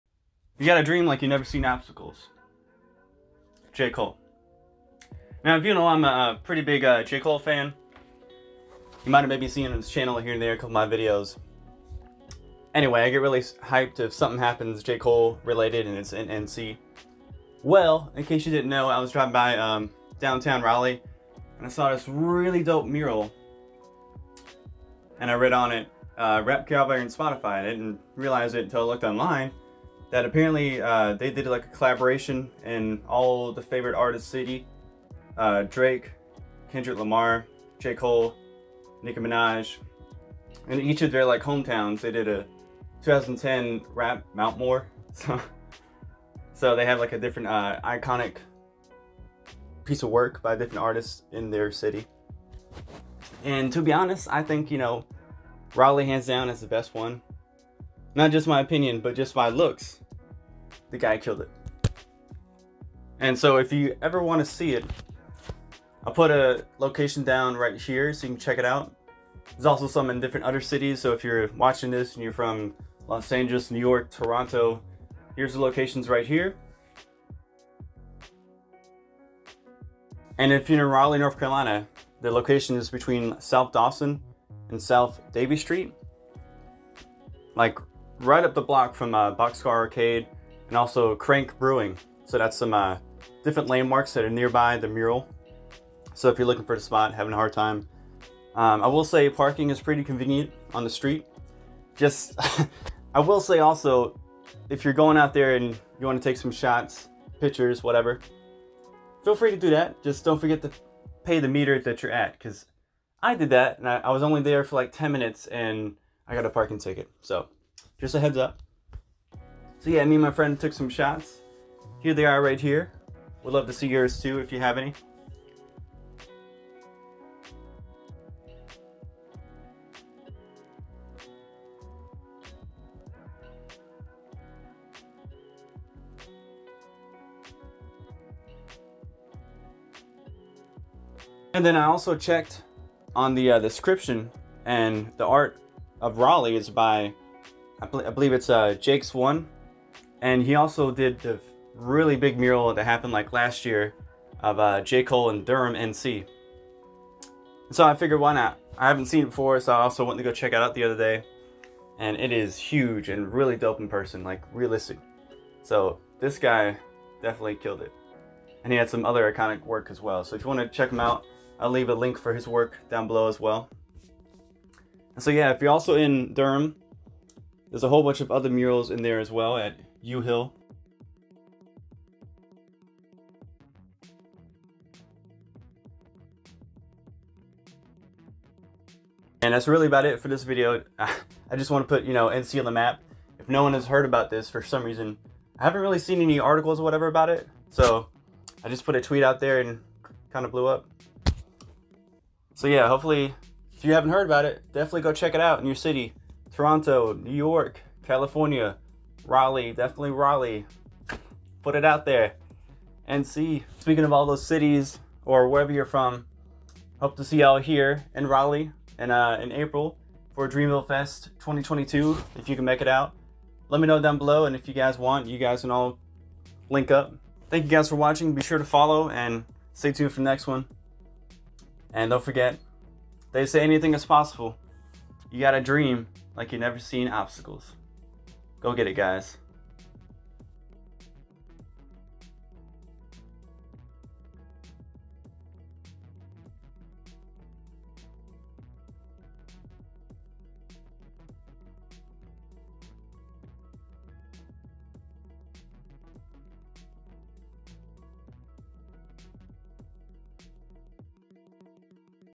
New Murals of 2010s Mt. Rapmore - Raleigh, LA, NYC, Toronto- ft Drake J cole Kendrick Lamar & Nicki
Have you heard of the 2010 mt. rap-more tribute to the legends that inspired many - kendrick Lamar, J. Cole, Drake, & Nicki Minaj?
 In collaboration between rap caviar & Spotify, you can find murals of these hip hop icons through NYC, LA, Raleigh & Toronto - their hometowns!

Shoutout to my friend Dylan for getting me some dope shots in front of the mural !

I may be bias, but I think Raleigh had the best mural!

New York:
57 Kenmare st

Raleigh:
416 S Dawson Street
Raleigh NC 27601 (downtown raleigh between S. Dawson & S. Davie street, near crank brewing)

Durham Murals:
3806 University Dr.

Toronto:
224 Queen St. W
Toronto, ON M5V 1Z6, Canada